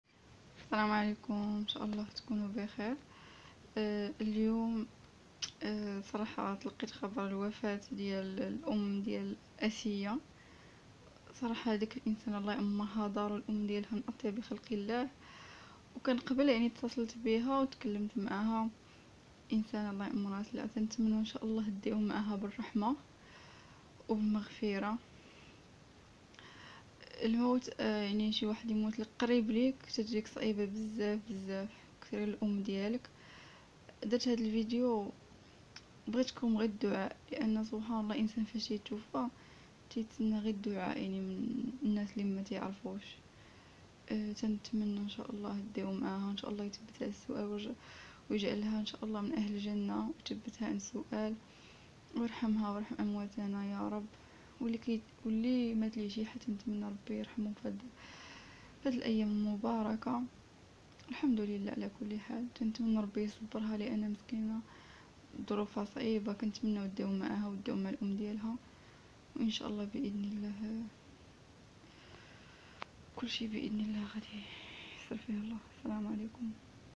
خبر وفاة صدمة 😖 assia Beauty الله يصبرها ياارب و اجعلها من اهل جنةهاد سيدة دارت خير دعيو مع مها 💔
دعيو. معاها . موت الأم صعيب و هي مسكينة في حال يعلم بيها الله دعواتكم   .  إنسانة طيبة و اقل حاجة نديروها هو دعاء